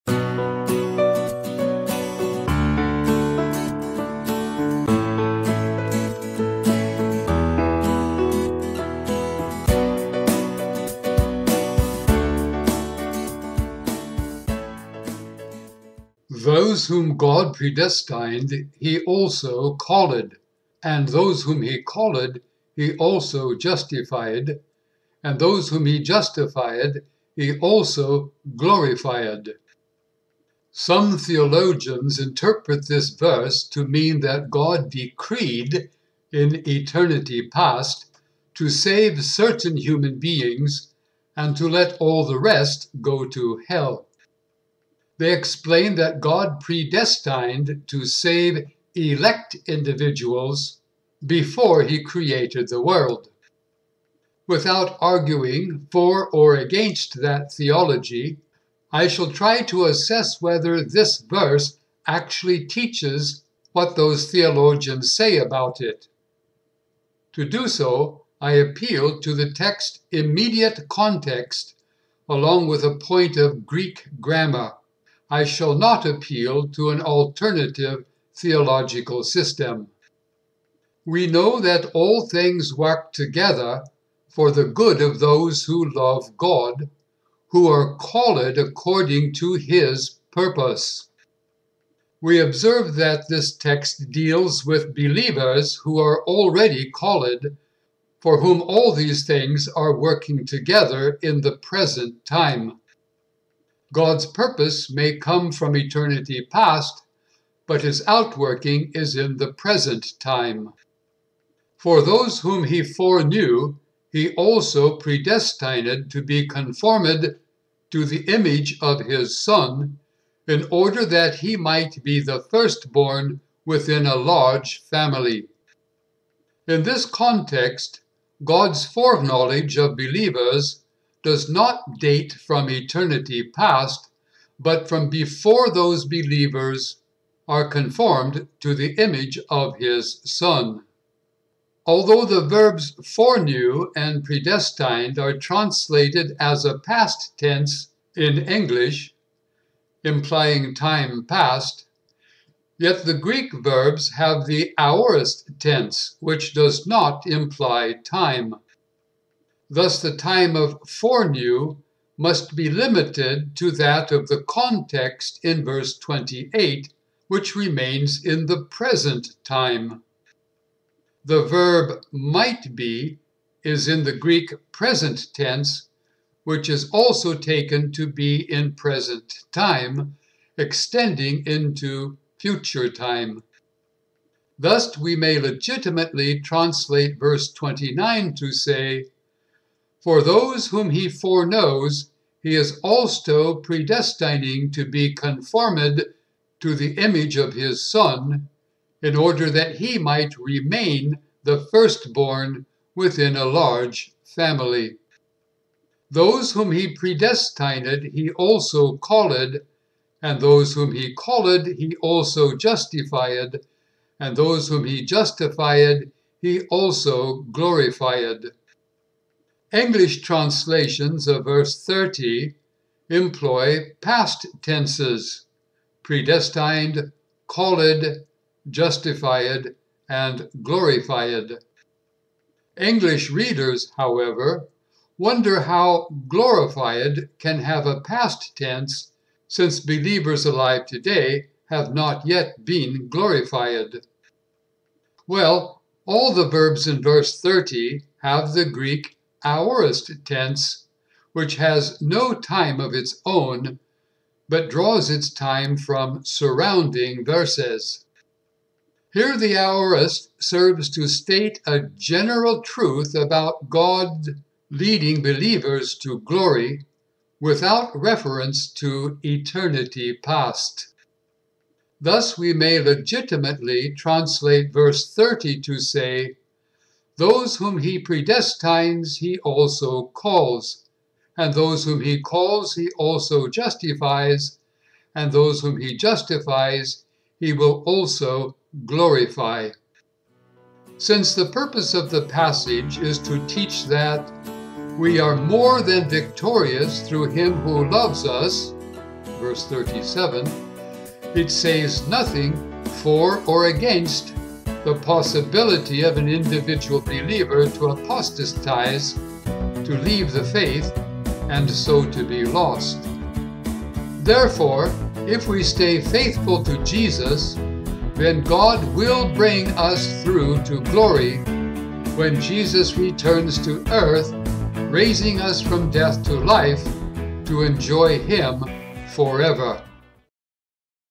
Predestination in Romans 8:30 (corrected)
INTRODUCTION
“Those whom [God] predestined he also called, and those whom he called he also justified, and those whom he justified he also glorified.” (NRSVue, 2022)
Some theologians interpret this verse to mean that God decreed, in eternity past, to save certain human beings, and to let all the rest go to hell. They explain that God “predestined” to save “elect” individuals before he created the world.
Without arguing for or against that theology, I shall try to assess whether this verse actually teaches what those theologians say about it. To do so, I appeal to the text’s immediate context, along with a point of Greek grammar. I shall not appeal to an alternative theological system.
CONTEXT AND GRAMMAR
28 We know that all things work together for good for those who love God, who are called according to his purpose.
We observe that this text deals with believers who are already called, for whom all these things are working together in the present time. God’s purpose may come from eternity past, but its outworking is in the present time.
29 For those whom he foreknew he also predestined to be conformed to the image of his Son, in order that he might be the firstborn within a large family.
In this context, God’s foreknowledge of believers does not date from eternity past, but from before those believers are conformed to the image of his Son.
Although the verbs “foreknew” and “predestined” are translated as a past tense in English, implying time past, yet the greek verbs have the aorist tense, which does not imply time. Thus, the time of “foreknew” must be limited to that of the context in verse 28, which remains in the present time. The verb “might be” is in the Greek present tense, which is also taken to be in present time, extending into future time.
Thus, we may legitimately translate verse 29 to say, “For those whom he foreknows he is also predestining to be conformed to the image of his Son, in order that he might remain the firstborn within a large family.”
30 Those whom he predestined he also called, and those whom he called he also justified, and those whom he justified he also glorified.
English translations of verse 30 employ pasts tenses: predestined, called, justified and glorified. English readers, however, wonder how glorified can have a past tense, since believers alive today have not yet been glorified.
Well, all the verbs in verse 30 have the Greek aorist tense, which has no time of its own, but draws its time from surrounding verses. Here the aorist serves to state a general truth about God leading believers to glory, without reference to eternity past.
Thus, we may legitimately translate verse 30 to say, “Those whom he predestines he also calls, and those whom he calls he also justifies, and those whom he justifies he will also glorify.”
CONCLUSION
Since the purpose of the passage is to teach that “we are more than victorious through him who loved us” (v.37), it says nothing for or against the possibility of an individual believer to apostatize, to leave the faith, and so be lost.
Therefore, if we stay faithful to Jesus, then God will bring us through to glory, when Jesus returns to earth raising us from death to life, to enjoy him forever.